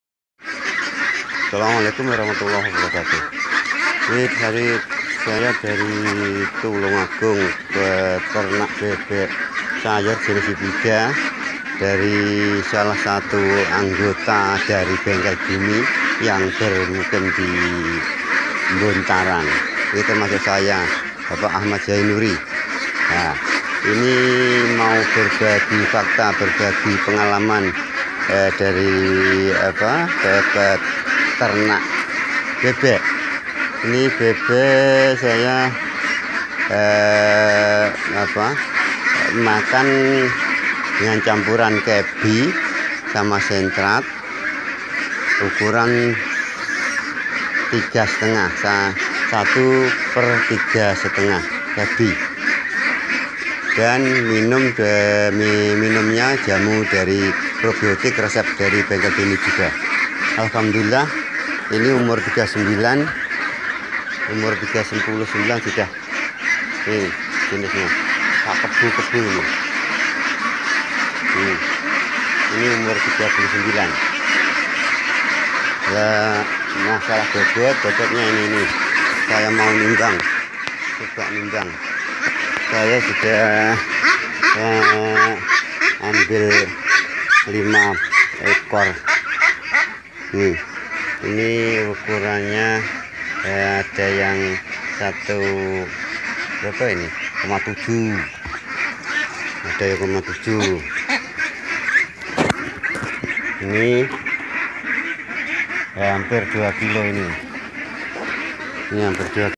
Bebek organik
Bebek pola bengkel bumi
Usia 39 hari ini
Bobot rata tembus 1,7 kg up

Pakan ngirit
Pertumbuhan ngacrit

kita mah gitu 😁😀😂